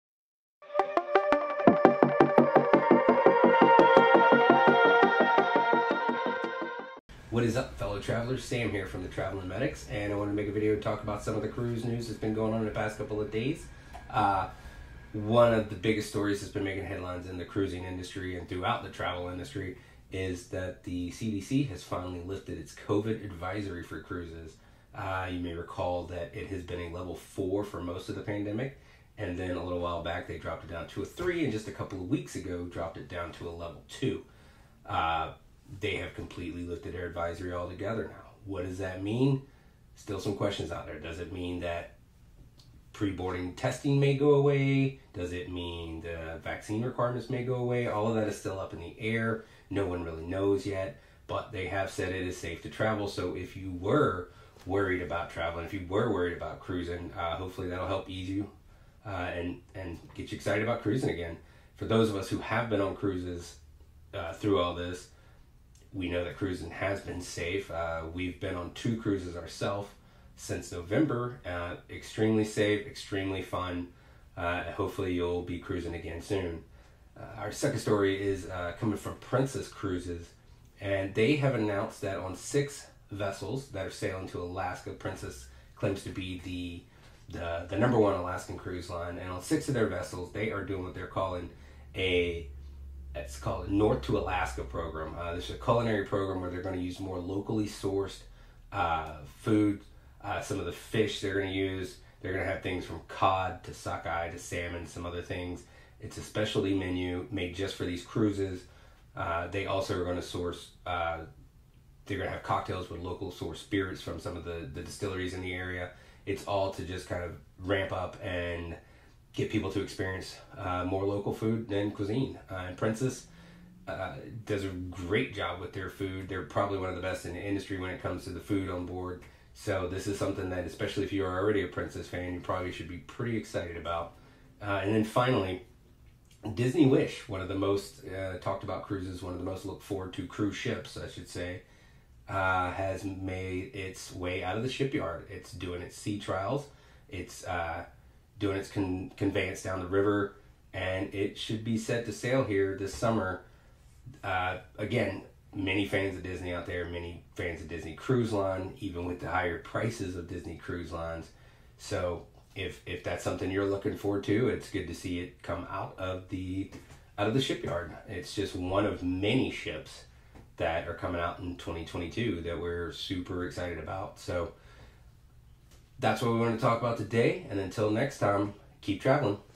Good Job CDC, Cruise News
CDC removes cruise warning, Princess adds a new specialty menu, and Disney Wish is in the news.